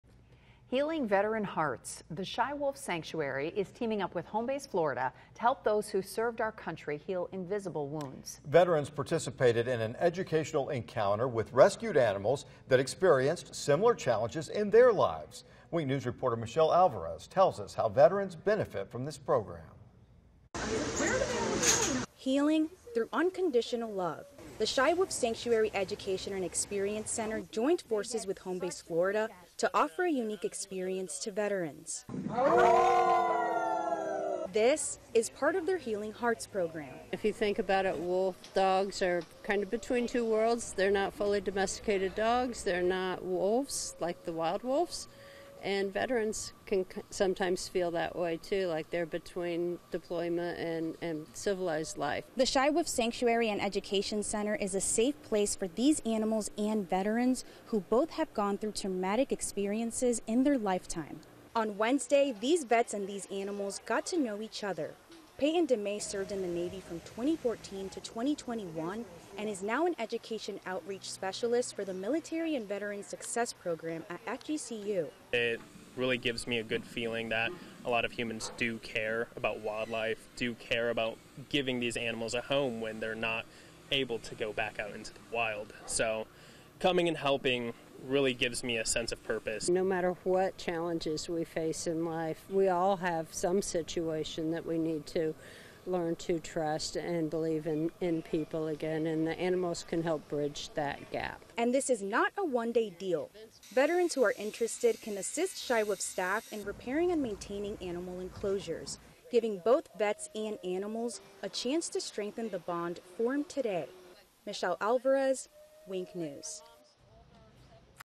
Shy Wolf Sanctuary and Home Base Florida helping veterans
The Shy Wolf Sanctuary is teaming up with Home Base Florida to help those who served our country heal invisible wounds